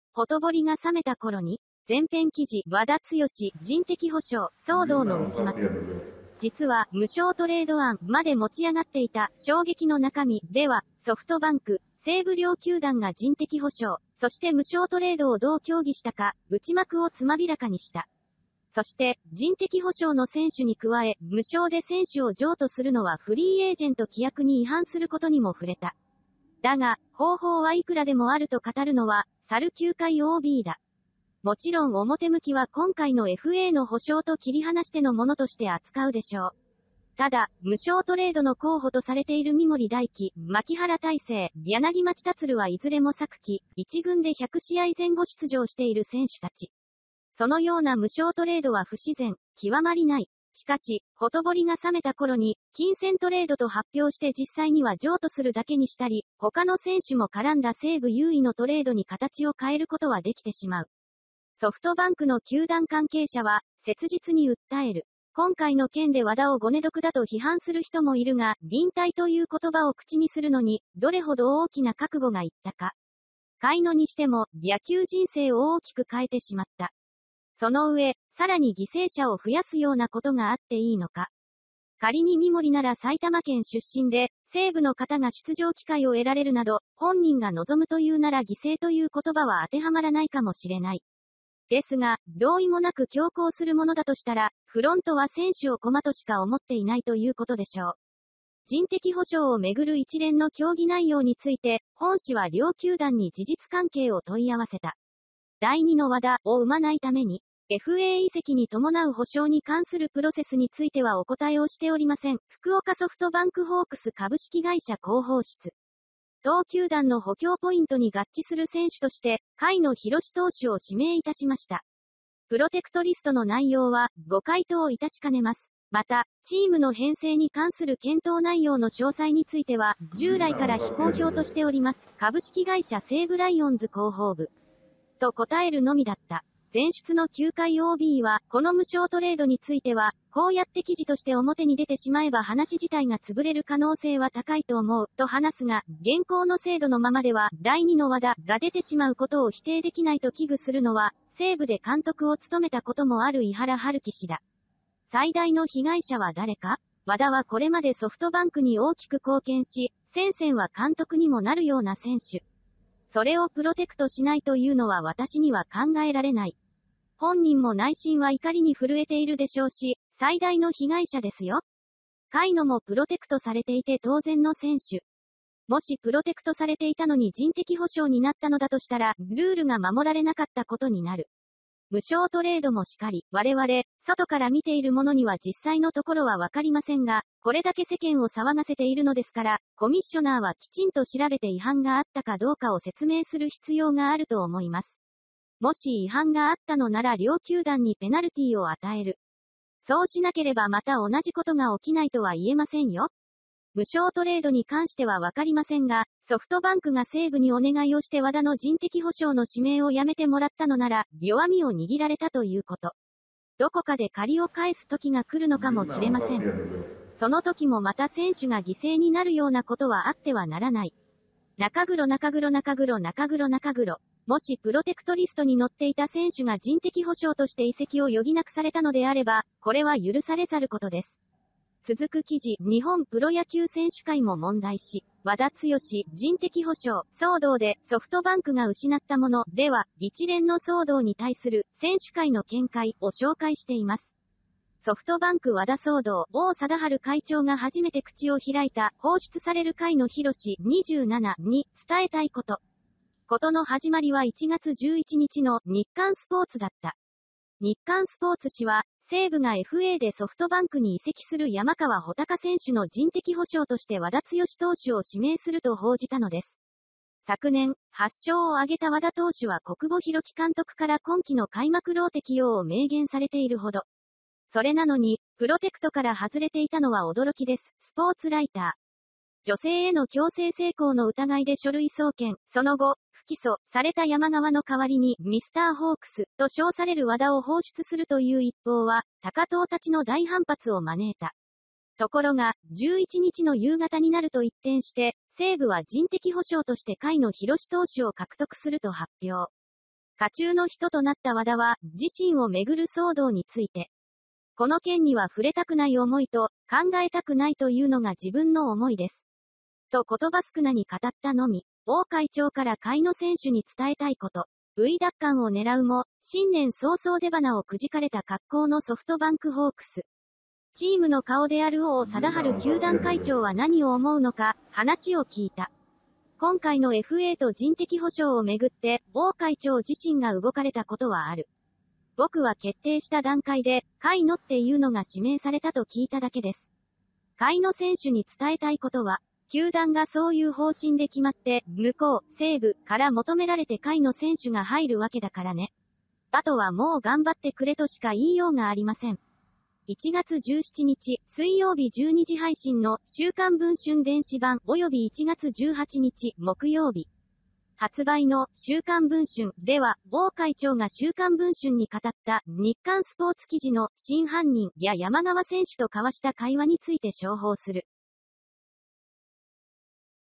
《独自》和田毅は「ゴネ得」なのか…ソフトバンクが西武に持ちかけていた、驚愕の「無償トレード案」の行く末
ほとぼりが冷めた頃に…

　前編記事『和田毅「人的補償」騒動の内幕…じつは「無償トレード案」まで持ち上がっていた「衝撃の中身」』では、ソフトバンク、西武両球団が人的補償、そして無償トレードをどう協議したか、内幕を詳らかにした。そして、人的補償の選手に加え、無償で選手を譲渡するのはフリーエージェント規約に違反することにも触れた。

　だが、方法はいくらでもあると語るのは、さる球界OBだ。

　「もちろん表向きは今回のFAの補償と切り離してのものとして扱うでしょう。ただ、無償トレードの候補とされている三森大貴、牧原大成、柳町達はいずれも昨季、1軍で100試合前後出場している選手たち。そのような無償トレードは不自然、極まりない。

　しかし、ほとぼりが冷めた頃に、金銭トレードと発表して実際には譲渡するだけにしたり、ほかの選手も絡んだ西武優位のトレードに形を変えることはできてしまう」

　ソフトバンクの球団関係者は、切実に訴える。

　「今回の件で和田をゴネ得だと批判する人もいるが、引退という言葉を口にするのに、どれほど大きな覚悟が要ったか。甲斐野にしても、野球人生を大きく変えてしまった。その上、さらに犠牲者を増やすようなことがあっていいのか。